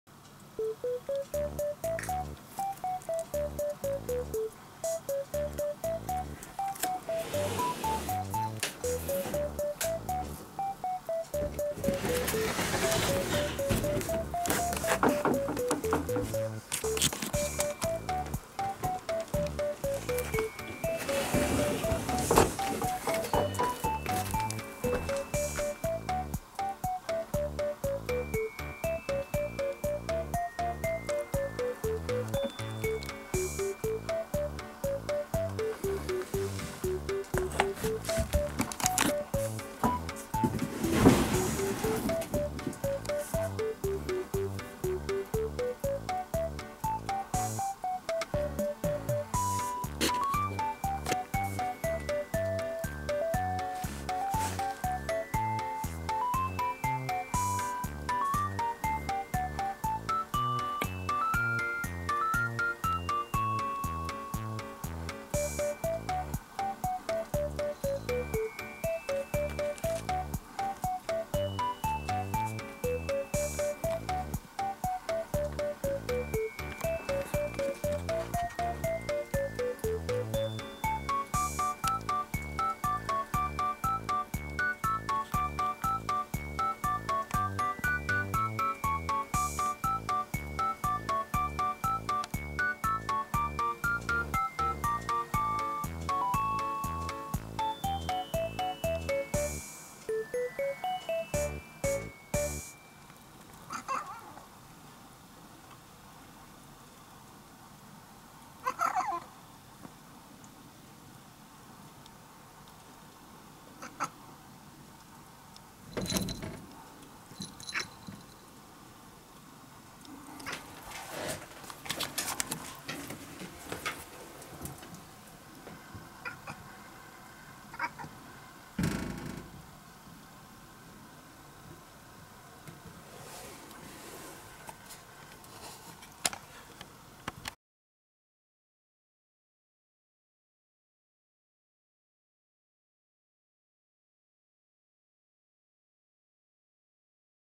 急な積雪でも平常心を崩さないキジトラさん。猫さんは何かにざわめく
何を考えているかわからないキジトラさん。
生きることしか考えてないのか？